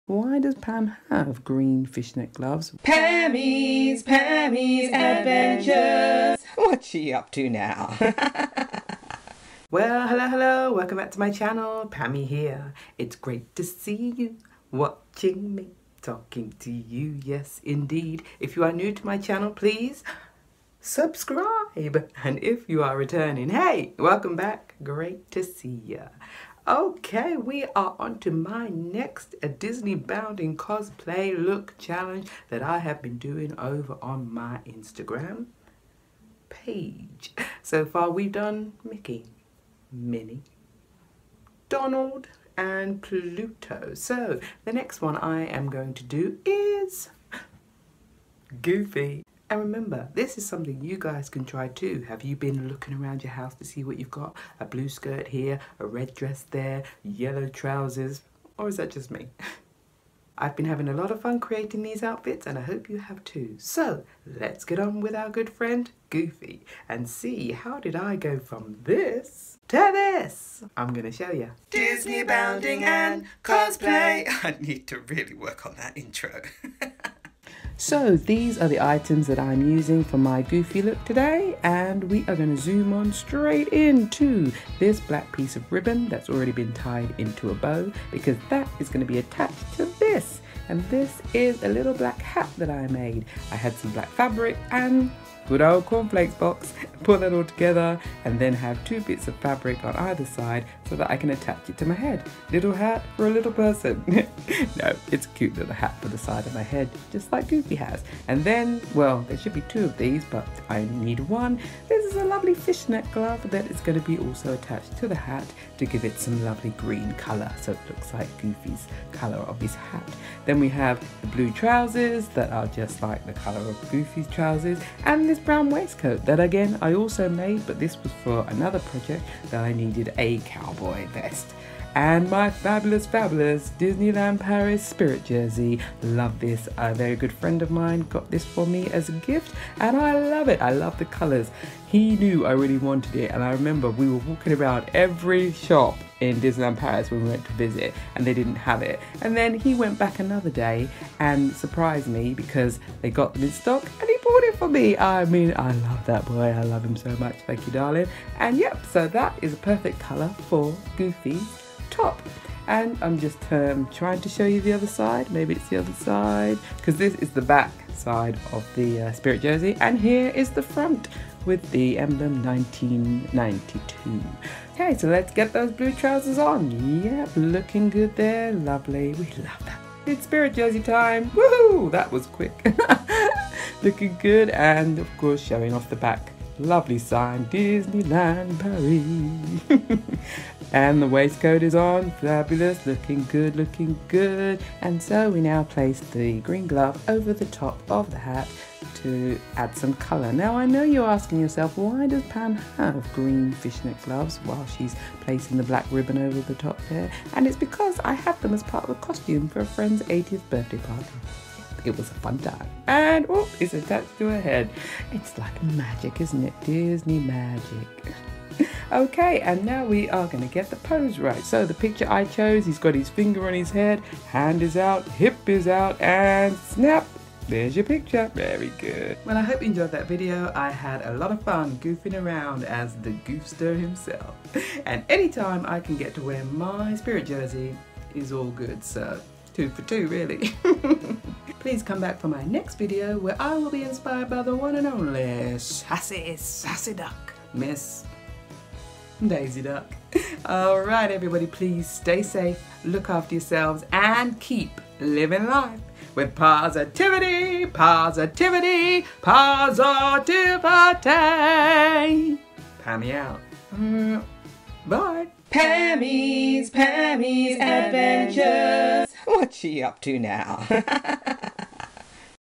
Goofy | How To Disneybound Cosplay Goofy On A Budget
Goofy | How To Disneybound Cosplay Disney Character Goofy On A Budget

I am having a lot of fun going through my wardrobe and finding items to create some bounds and cosplay outfits for Goofy

Keep living life with Positivity, positivity, positivity

I do not own the Goofy image